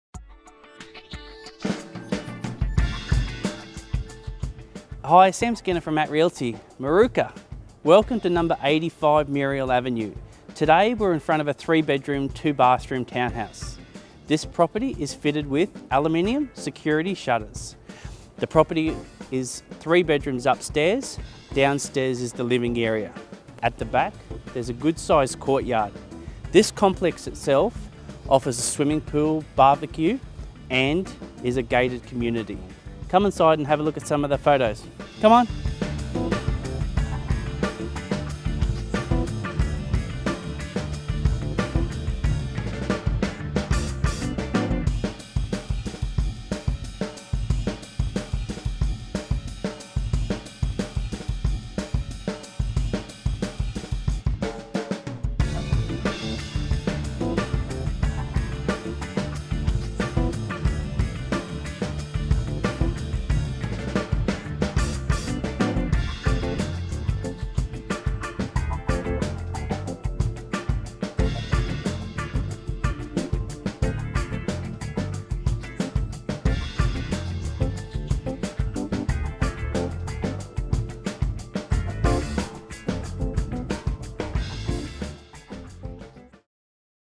Sam Skinner is proudly to represent 30/85 Muriel Av, Moorooka by @ realty
This brick and tile townhouse is fitted with aluminium secure fitted shutters (key lock) allowing you to leave doors and window open during the day, enjoying the breeze and privacy as well as offering a more modern look inside and out.

This property has had 2/3 of the garage converted to a great office space for working from home, a rumpus room great for entertaining kids, or just a possibly second living space.
This home is fitted with air conditioning upstairs in bedrooms and downstairs in living.
A combined lounge dining and wrap around kitchen which overlooks a private rockwall courtyard.

This property and complex was built by Phillip Usher the developer, who still in the industry.
Less than 3 a year in this complex come available in this facility.

Does it have side access for prams and bikes - Yes.
Is it walking distance to buses - Yes ( bus at doorstep to Indooroopilly and bus for City on Beaudesert Road.- 5 min walk)
Is there a termite treatment prevention in place - Yes.
Is there a Swimming pool and BBQ for the summer - Yes. This shared facility is located at front along with a private shared grassed area.
Security - Yes. Electric gates and onsite manger looking after the complex and the gardens.
Third toilet off the downstairs laundry - Yes
Visitor parking - Yes

All this and more should be making you say YES - Let's call Sam Skinner today! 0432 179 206